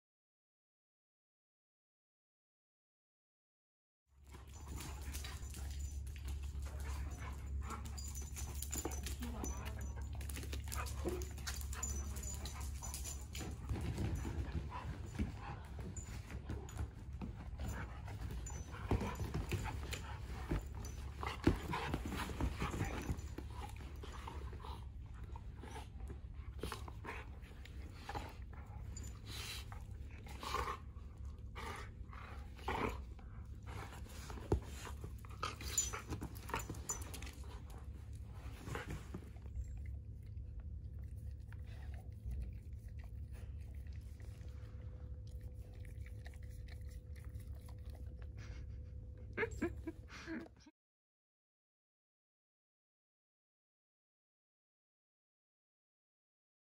Good boys Sako and Samson having fun | Doggy Daycare
Sako and Samson loves playing. If you watch all the way to the end, you get to see how much these beautiful boys really love each other 😘🐾

Dog breeds featured in this video:
Samson the Bulldog and Sako the Viszla

For more videos with dog love